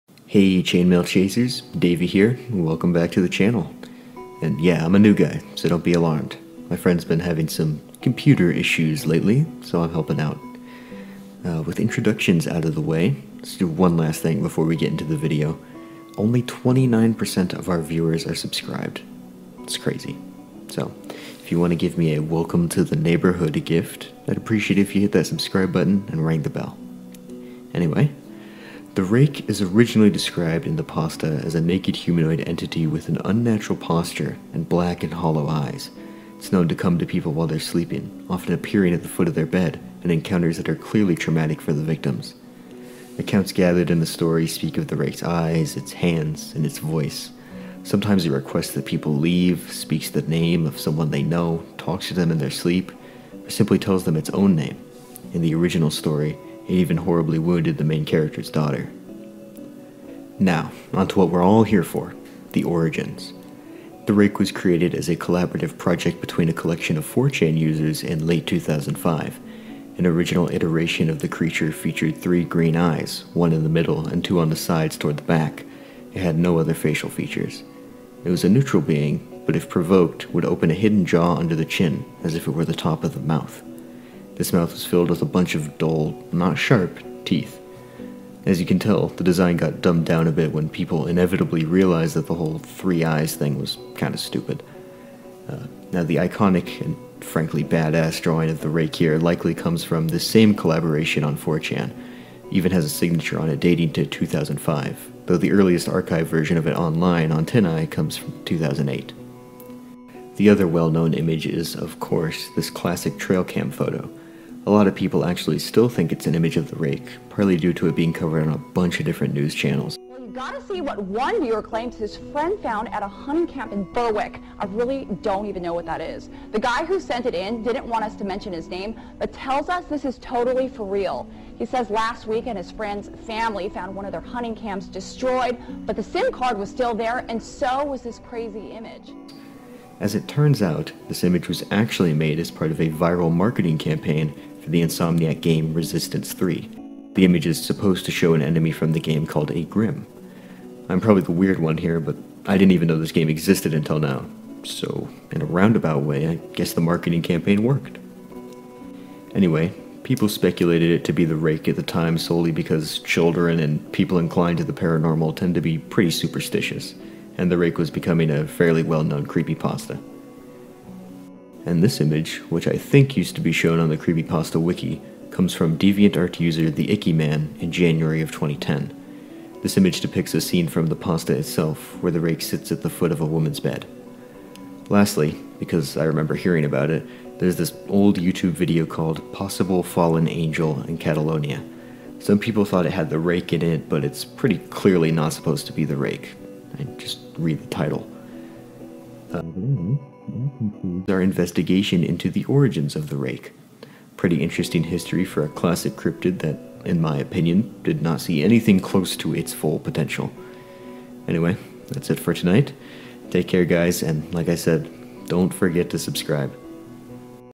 The Rake - Creepypasta Image Origins
This episode we look at various images associated with The Rake, hear some trivia about an earlier design, and meet a new narrator.

The Rake is the titular main antagonist of the Creepypasta story of the same name. It is a humanoid creature with large teeth and claws that stalks it's victims and appears at the end of their beds as they sleep, or in the woods or in their houses.
Since 2006, pictures, fan-art and video blogs have been made of the monster, including numerous allusions in the Slenderverse YouTube series EverymanHYBRID